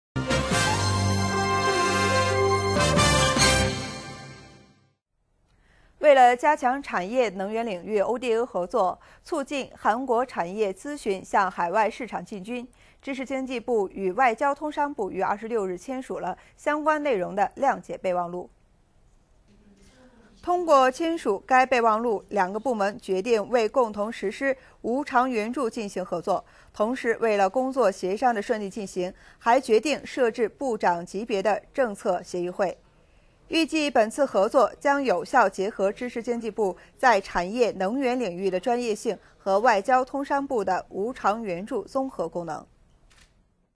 加强产业能源ODA合作
为了加强产业能源领域ODA(官方开发援助)合作，促进韩国产业咨询向海外市场进军，知识经济部与外交通商部于26日签署了相关内容的谅解备忘录。

通过签署该备忘录，两个部门决定为共同实施无偿援助进行合作，同时，为了工作协商的顺利进行，还决定设置部长级别的政策协议会。

预计本次合作将有效结合知识经济部在产业能源领域的专业性，和外交通商部的无偿援助综合功能。